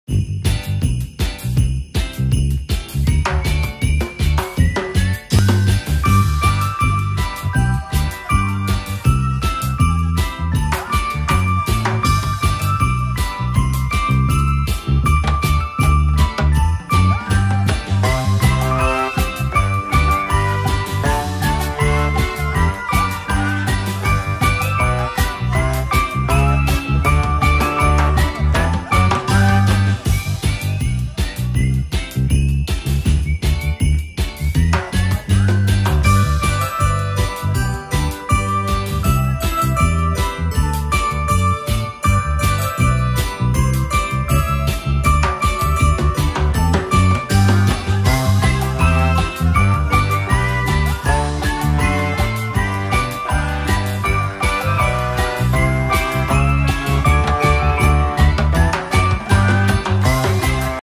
Otero County holiday display is sure to delight
This display in High Rolls, New Mexico goes above and beyond.